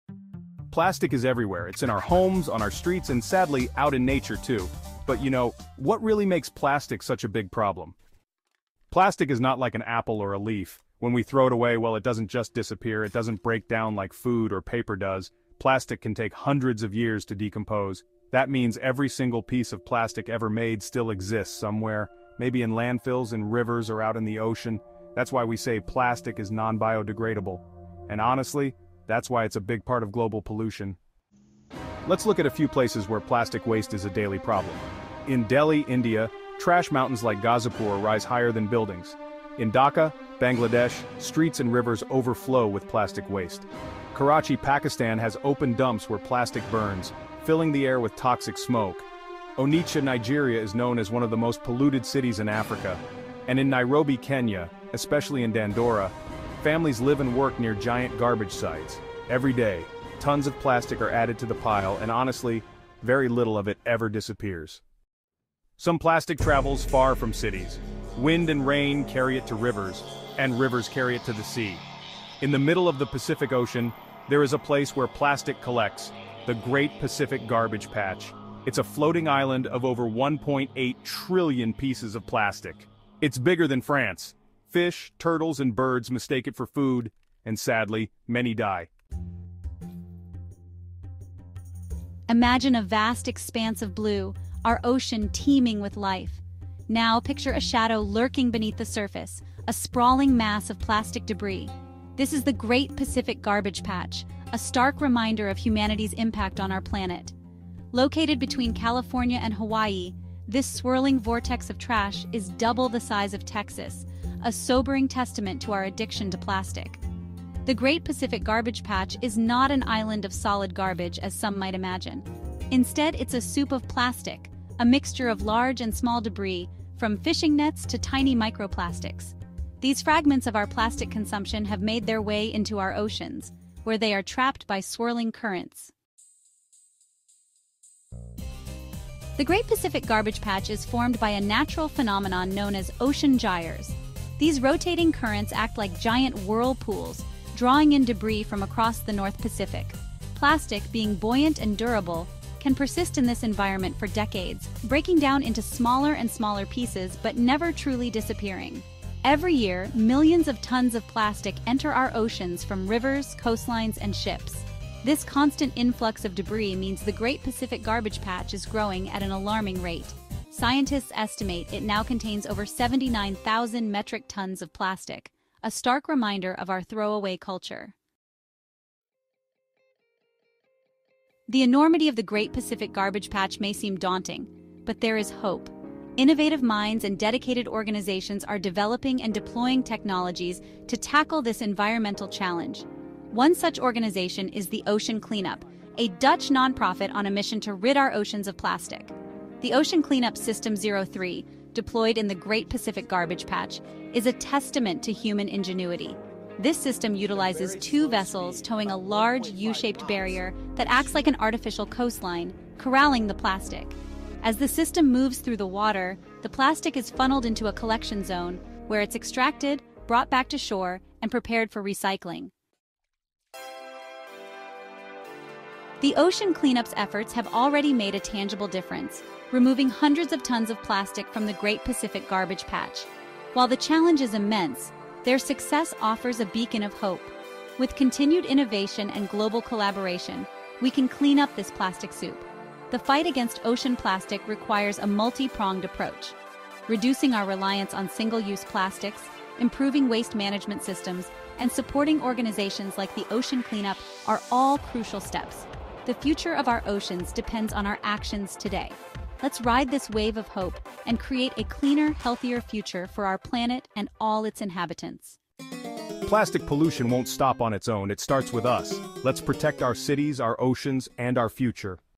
"Plastic Apocalypse: The Truth About the Great Pacific Garbage Patch"
🌊 What is the Great Pacific Garbage Patch?
Every year, millions of tons of plastic end up in our oceans — but where does it all go? In this eye-opening video, we explore the shocking reality of plastic pollution and uncover the truth about the Great Pacific Garbage Patch, the largest accumulation of ocean plastic in the world.

📍 Located between California and Hawaii, this floating "island" of plastic waste is three times the size of France. It’s not just plastic bags and bottles — it’s ghost nets, microplastics, and invisible toxins that are destroying marine life and poisoning our planet.

🎥 Through real footage, animations, and expert insights, we’ll take you on a visual journey into one of the most devastating environmental disasters of our time.

🔍 In this video you will learn:

What causes the Great Pacific Garbage Patch

How plastic affects sea life, ecosystems, and human health

What countries and individuals can do to help

Simple actions YOU can take starting today

🧠 Perfect for students, educators, and eco-conscious viewers, this video is a must-watch if you care about the future of our planet.